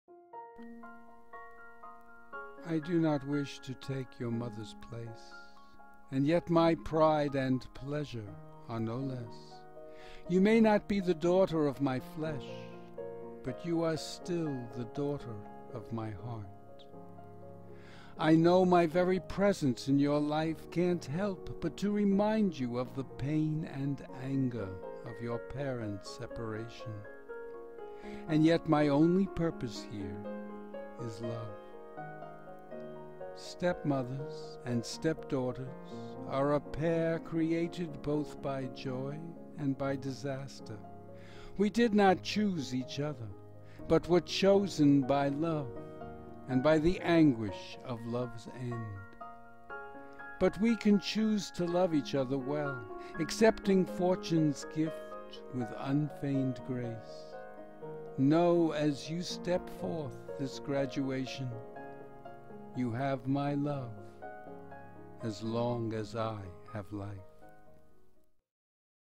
I Do Not Wish to Take Your Mother's Place
A graduation poem from a stepmother.